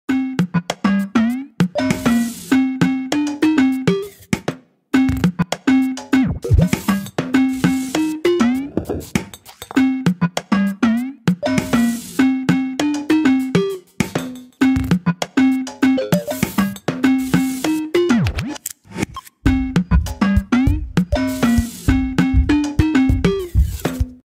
Dog Blessing Other Dogs 🤣🤩. This dog is like a God For other Dogs
Here this dog is like a God for all other dogs😱. Dog is blessing all dogs in his way.
Give a like now for this dog and share to make video viral.

ByBy.